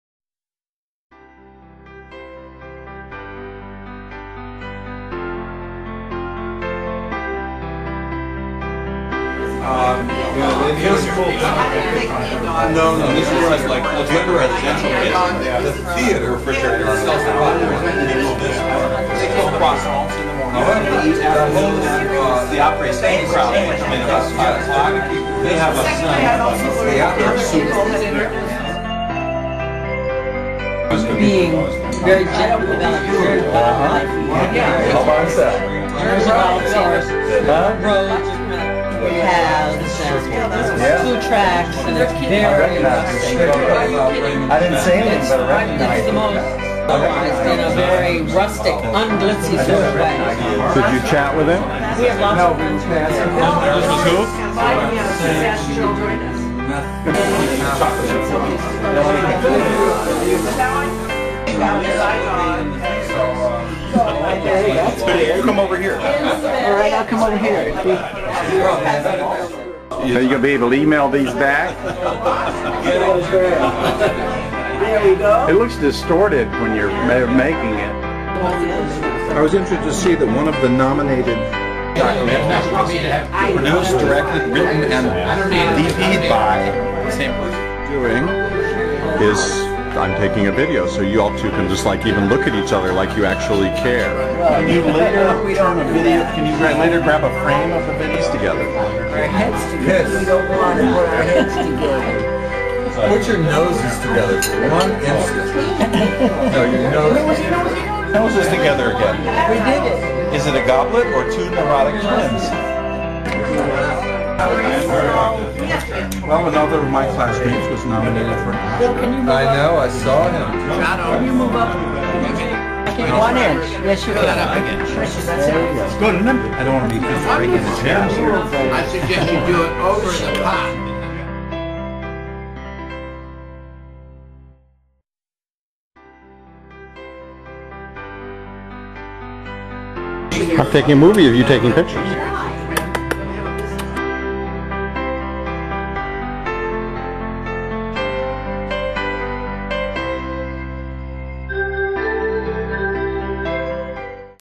Mary and Brownie's 85th Birthday
Home video of Brownie Borden and Mary Griggs 85th birthday with the family, March 2006